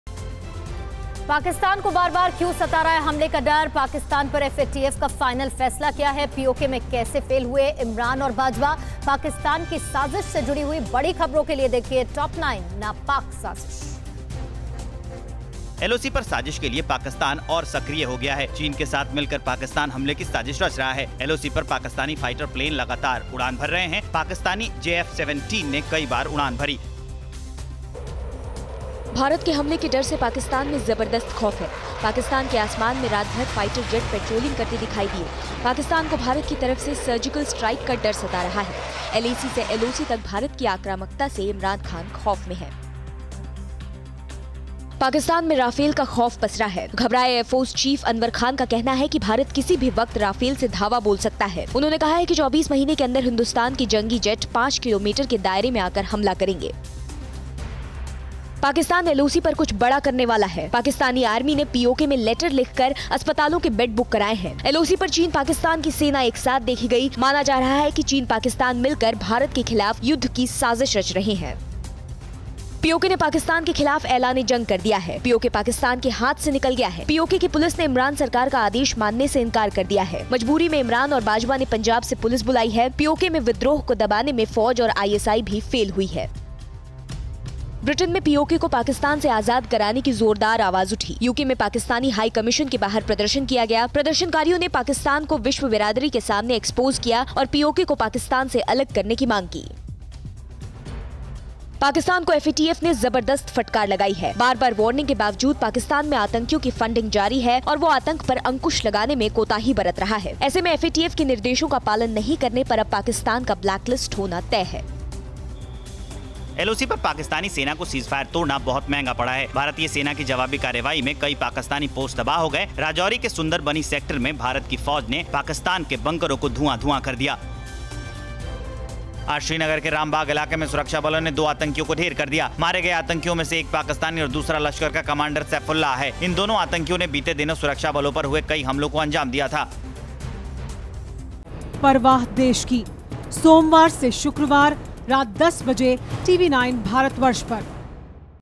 News Top 9 ना'पाक' साजिश : राफेल से हमला कर सकता है भारत- पाकिस्तान
-LoC पर साजिश के लिए और सक्रिय हुआ पाकिस्तान
-भारत के हमले के डर से पाकिस्तान में जबरदस्त खौफ
-राफेल से हमला कर सकता है भारत: पाकिस्तान
-PoK में पाक आर्मी ने अस्पतालों के बेड बुक कराए
-पाकिस्तान में इमरान की बर्बादी पर सबसे बड़ी खबर